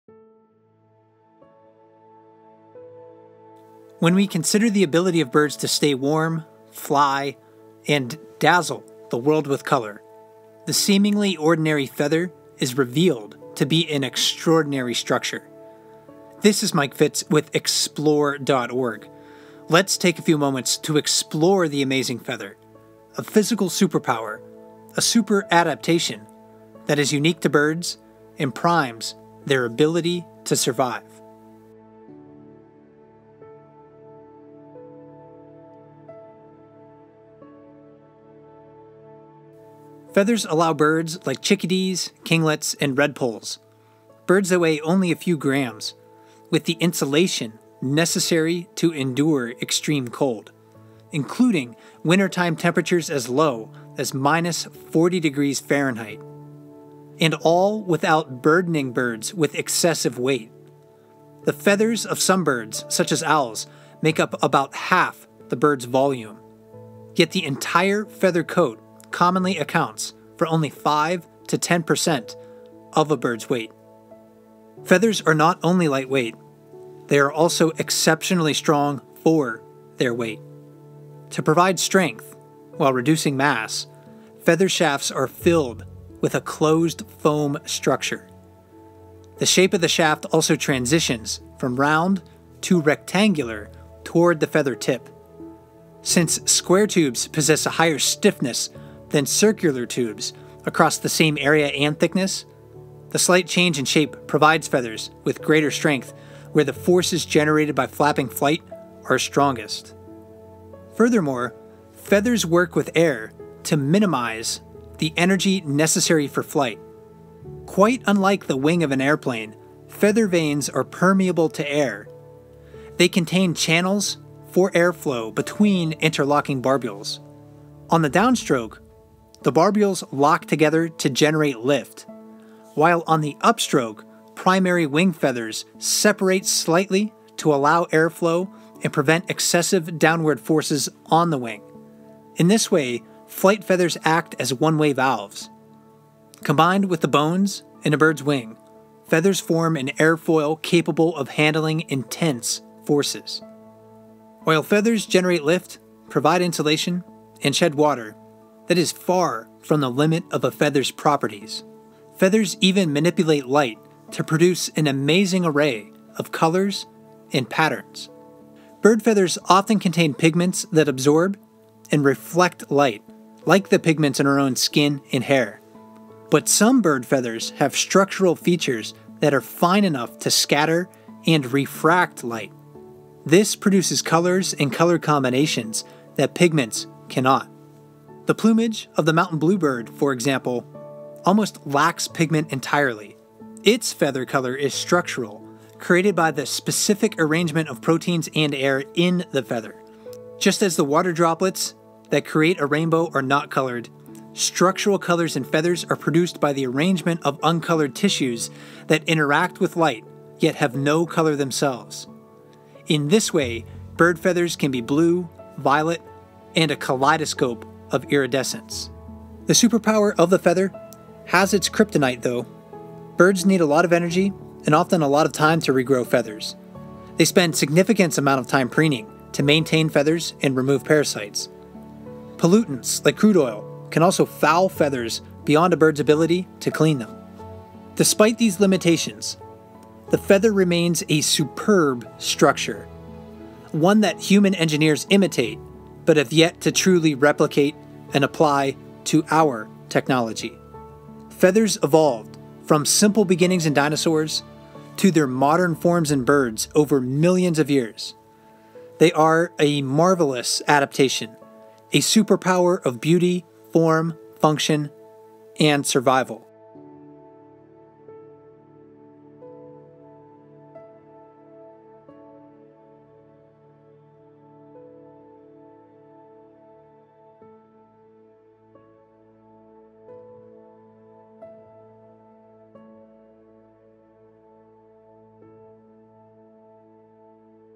Are Feathers a Bird Superpower? | Mike Fitz Explores
Mike Fitz joins us to explain how feathers are basically a super power.